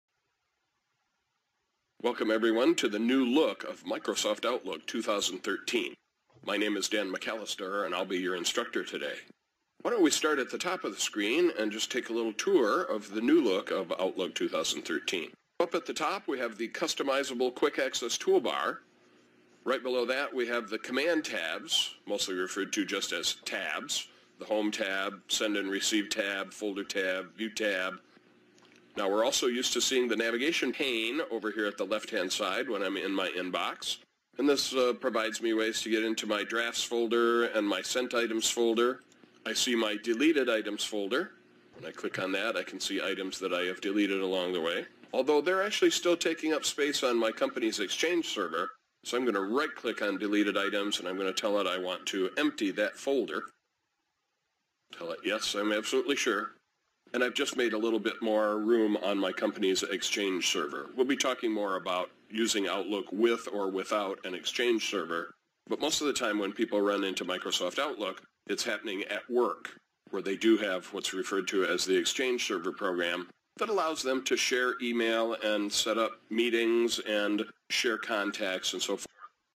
Outlook 2013 Training Course
Outlook is the most popular email and scheduler. It’s one of those “expected to know” programs. As well as manage email, it has the ability to also oversee your schedule, retain your contacts, and help keep your tasks on track. This course will teach you how to best utilize your office outlook